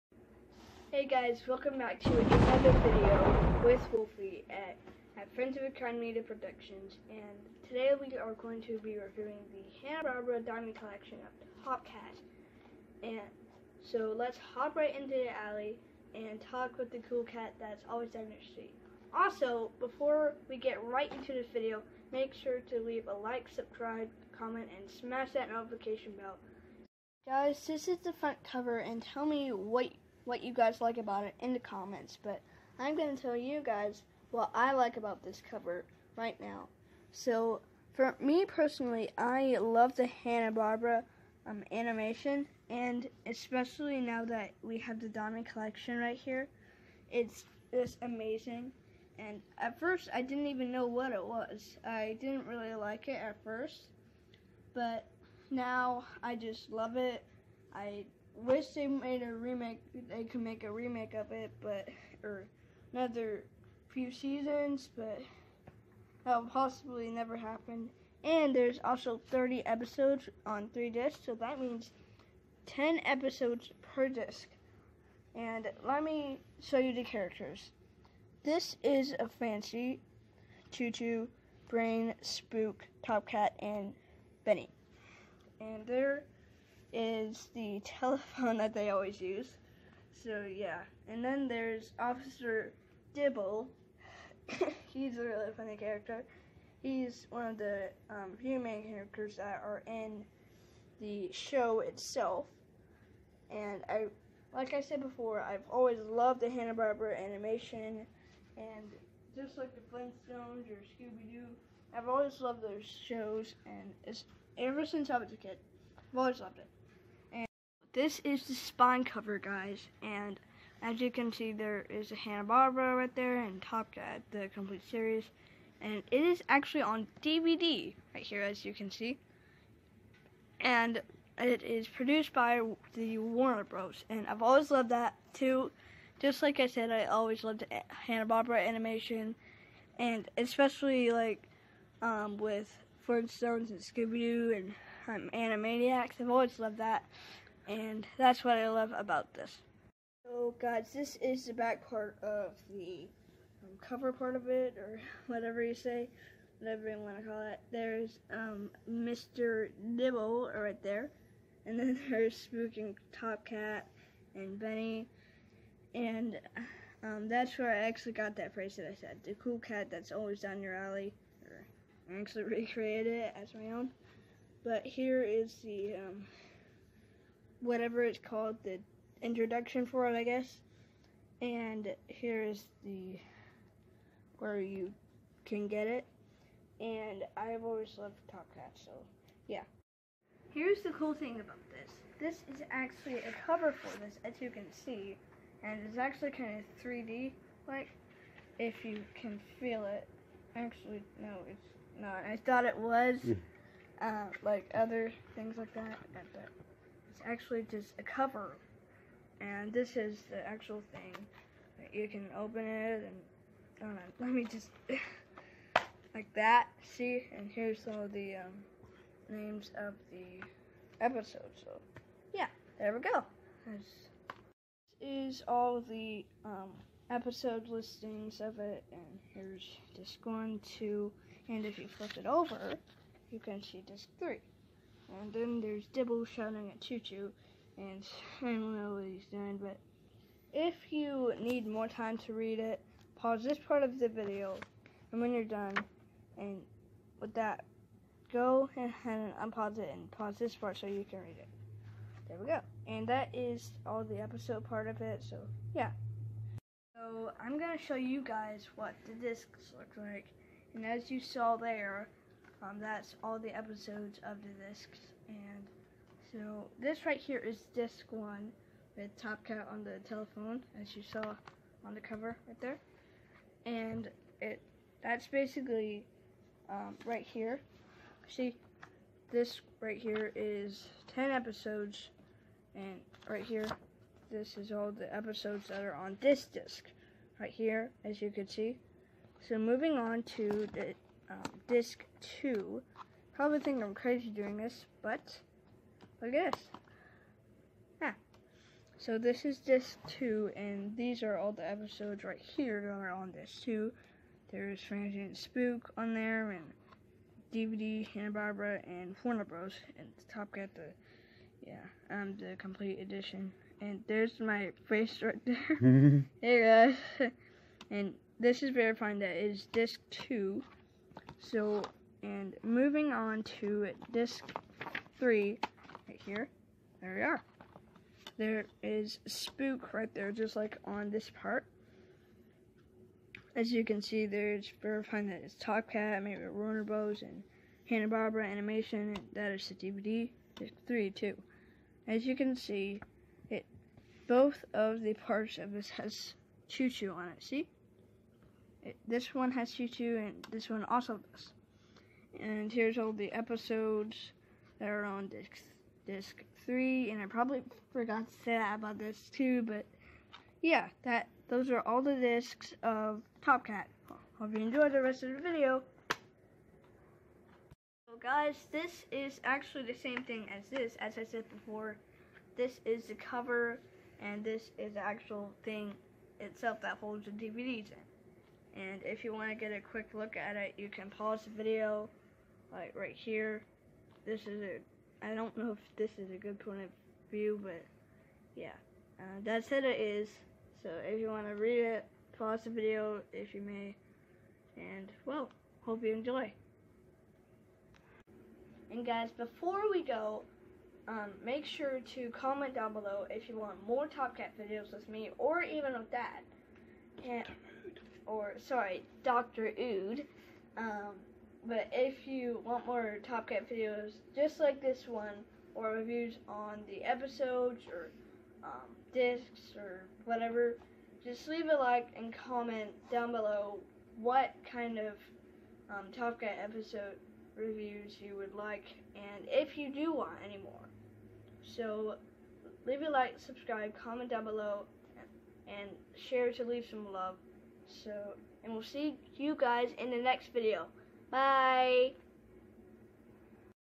Ood Delta Talks: Top Cat:- The Diamond Collection Box Set Review
Fan Review on Hannah Barbara's Top Cat, The Complete Series Box Set